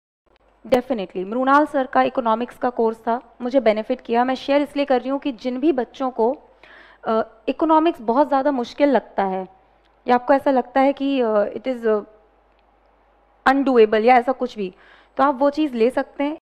Mrunal economy,#jagrti awasthi mam
Hey aspirants welcome to LBSNAA IAS mussoorie

in this video upsc topper jagrti awasthi mam says about sources of economy
here mam share about strategy
here mama suggest for best educater of economy

means- mrunal Patel sir
mrunal Patel sir is one of the best teacher for economy